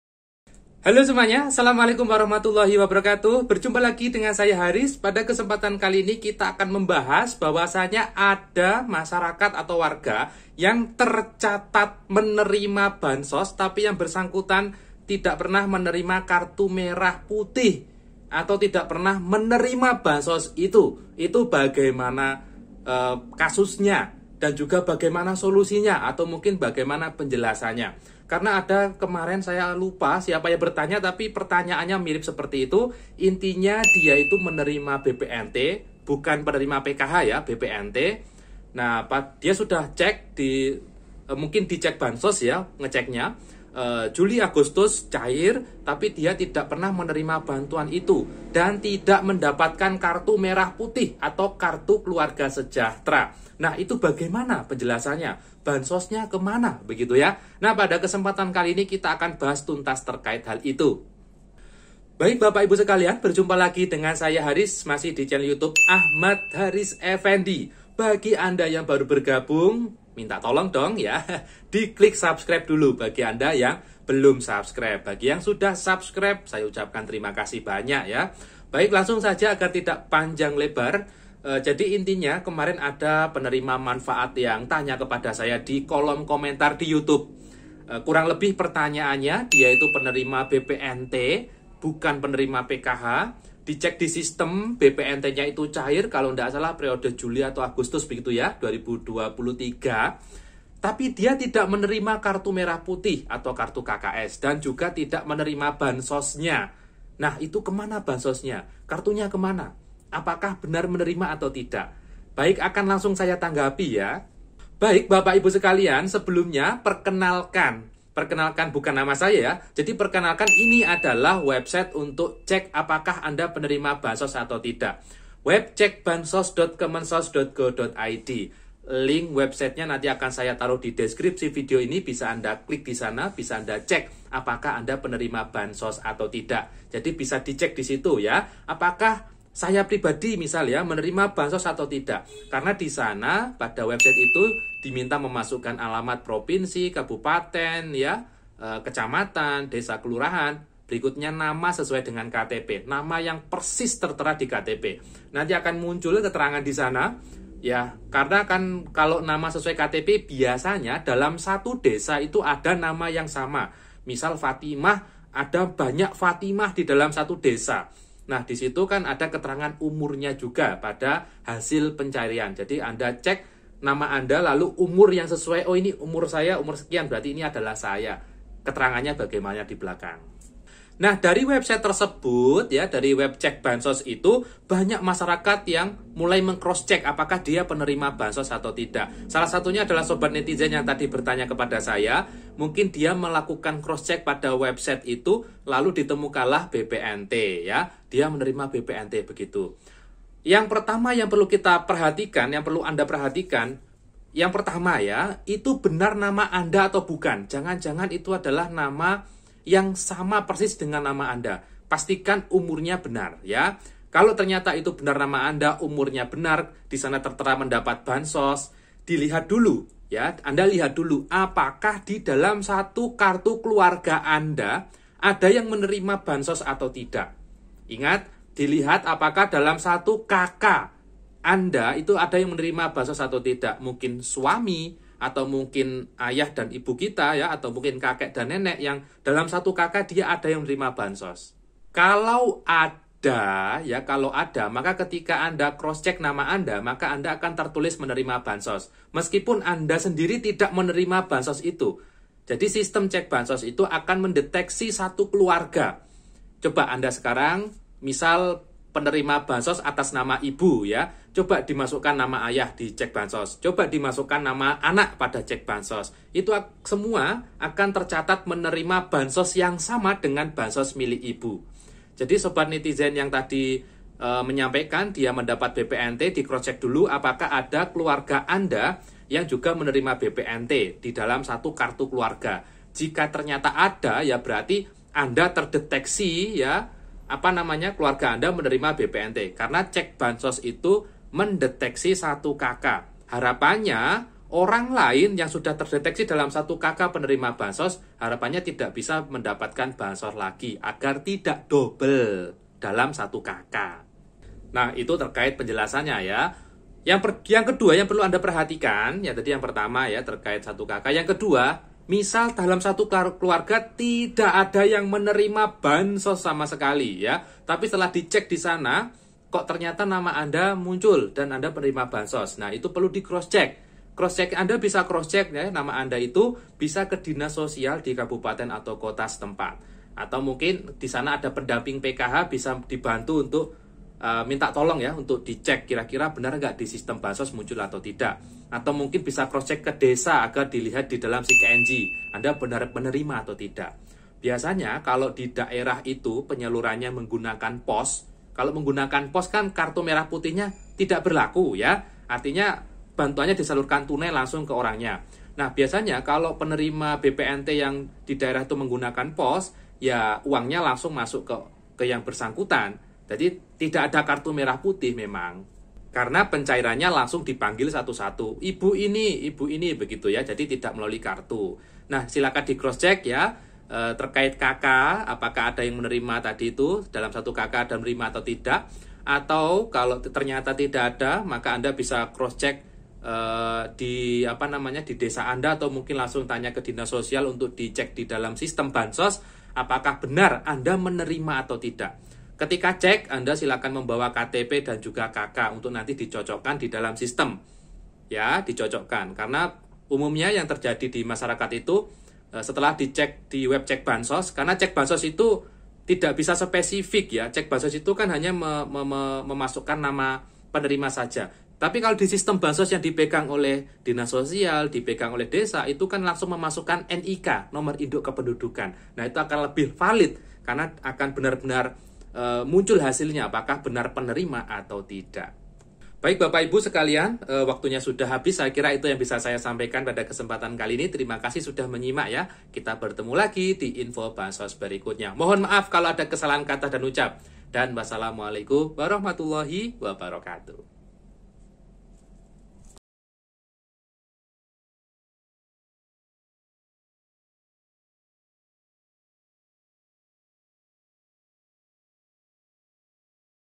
🔴GAWAT❗❗ TERCATAT MENERIMA "PKH/BPNT" TAPI TIDAK PERNAH TERIMA BANTUANNYA | MUNGKIN INI PENYEBABNYA
LINK CEK BANSOS